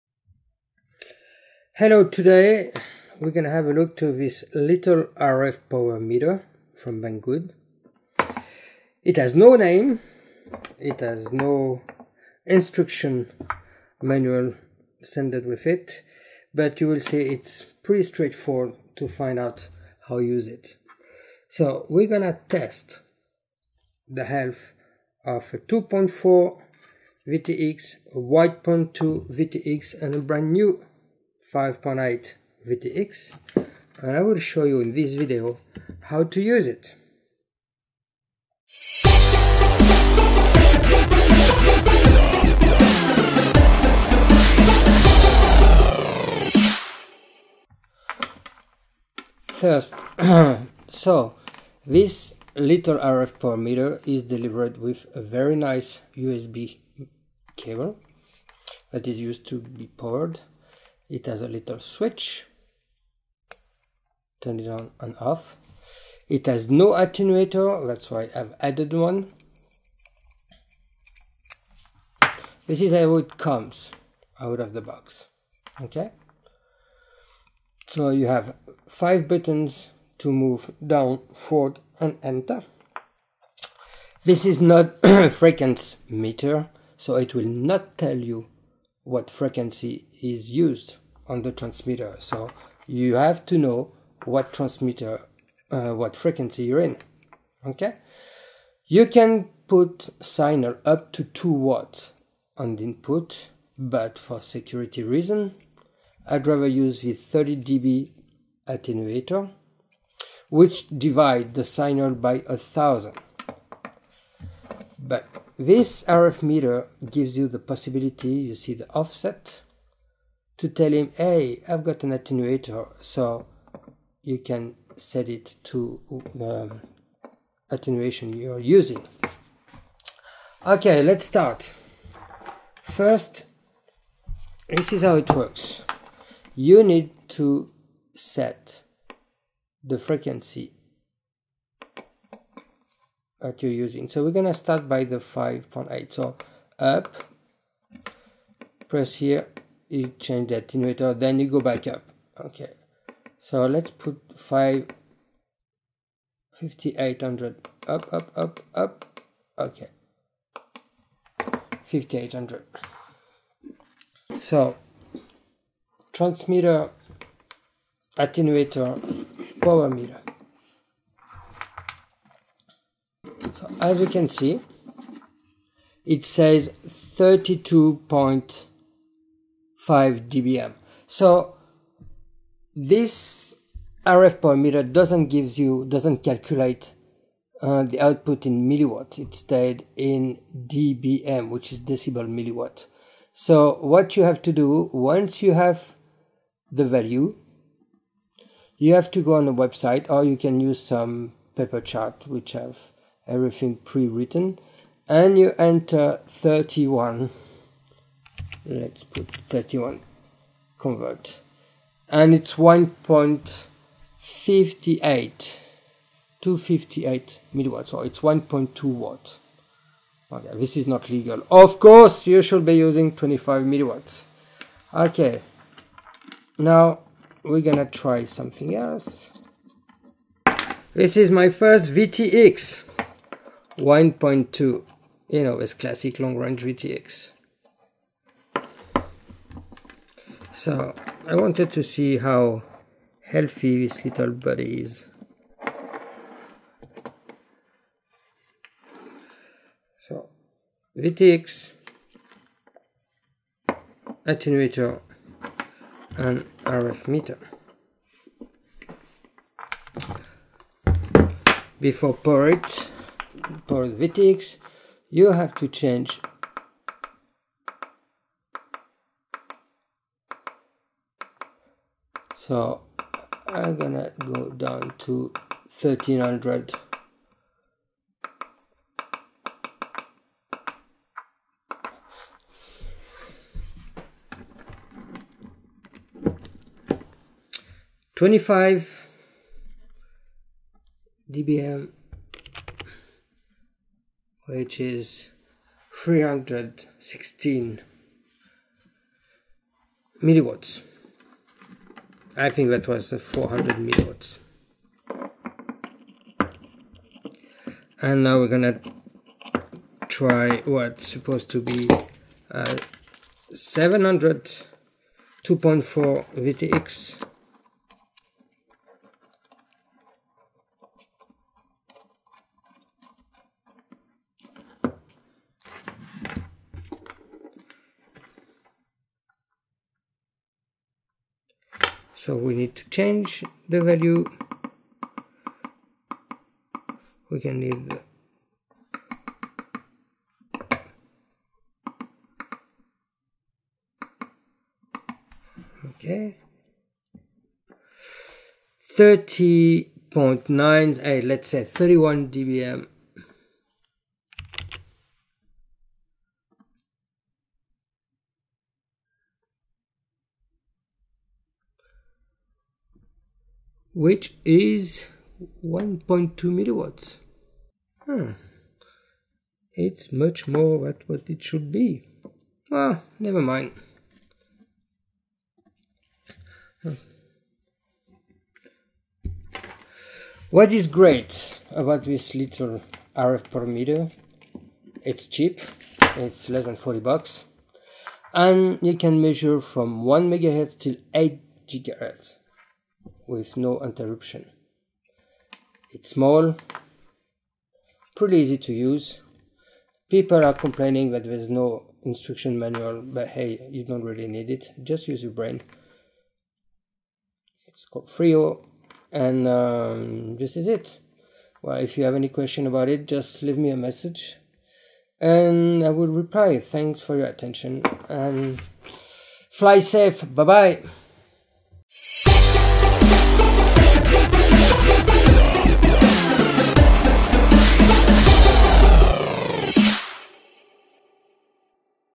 Bangood RF Power Meter RF-Power8000 1Mhz-8000Mhz OLED review and test
Test and review of this little RF POWER meter wich can go from 1mhz up to 8000 MHz !!!
A must have if you need to check the reliability of your VTX or check the output power of your Long Range equipement.
In big lines, this great rf metear is:

* * Cheap   40$
* * Color screen
* * Ajustable attenuator
* * Power Range mesure from 1nW to 2W
* * Power by micro USB (5V)